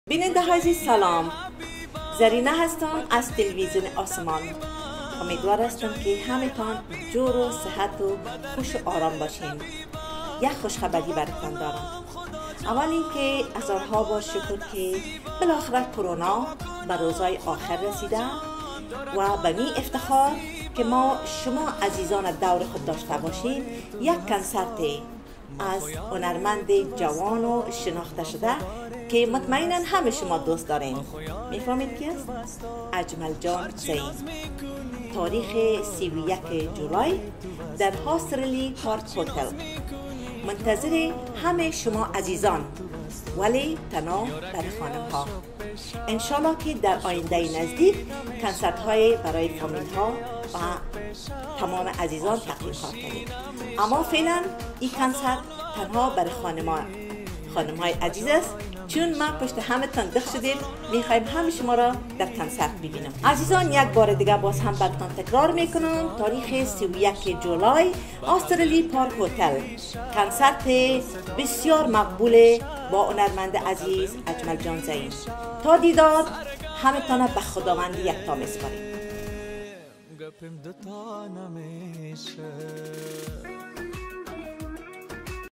Ajmal Zahin Concert in London 31st July 2021
مژده به خانم‌های هنردوست ؛
به مناسبت پایان یافتن خانه نشستن های دوامدار  بیایید در یک شب کاملا بیاد ماندنی دور هم بیاییم ، قصه های دوستانه سرکنیم  و از آهنگ های ناب هنرمند خوش آواز اجمل جان ذهین لذت ببریم ،